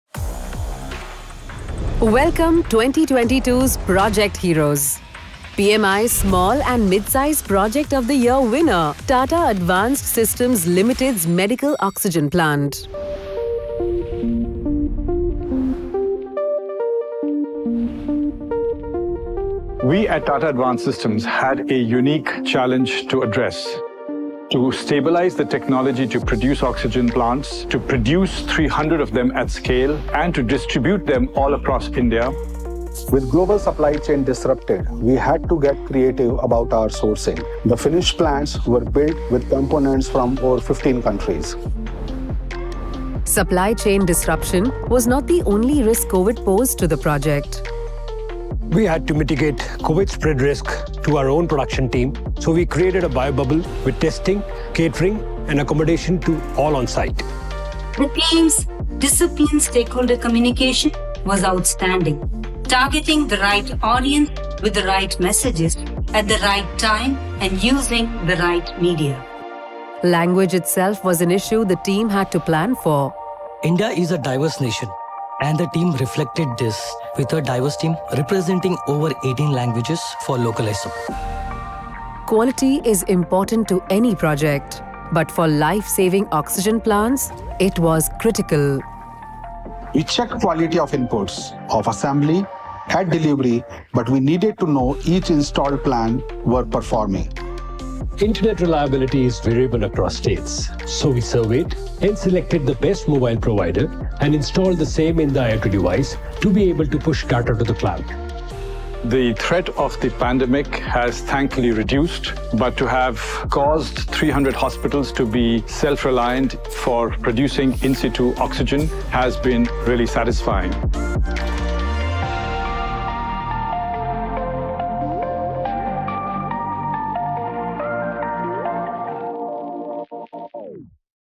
2022 PMI Small and Mid-Size Project of the Year: Medical Oxygen Plant by Tata Advanced Systems
The winner of the 2022 PMI® Small and Mid-Size Project Award was the Medical Oxygen Plant (MOP) by Tata Advanced Systems Limited (TASL).